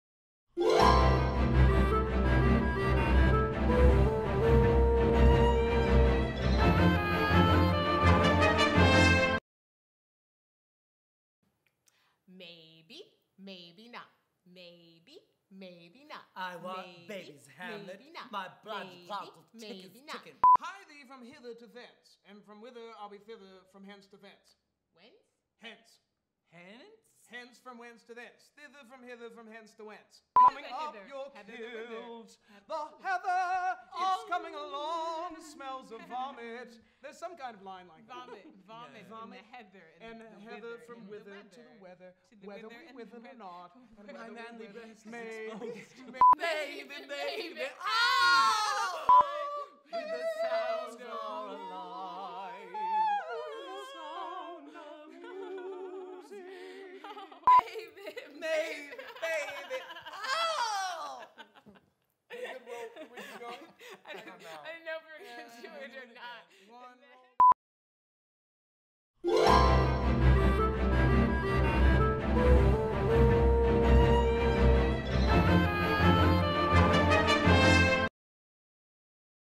Theatre Du Jour presents "The Complete Works of William Shakespeare (Abridged)" - Outtakes
The ensemble gets silly as they rehearse The Complete Works of William Shakespeare (Abridged) - Don't miss this show friends! July 15th at GS Steamers in Oswego, NY! Tickets available at dujourcny.com!